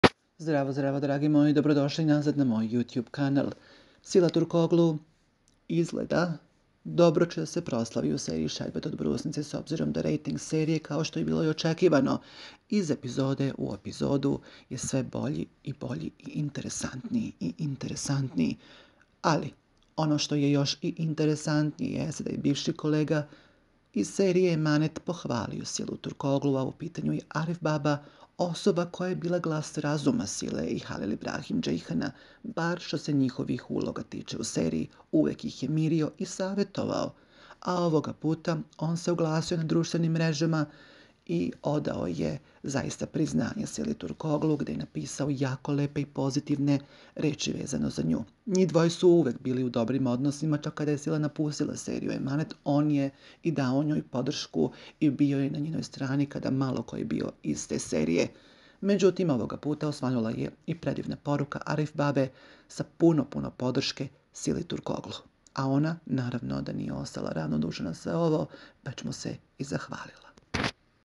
DESILO SE NEOCEKIVANO - Sila Turkoglu dobila poruku LJUBAVI od NJEGA
Sila dobila poruku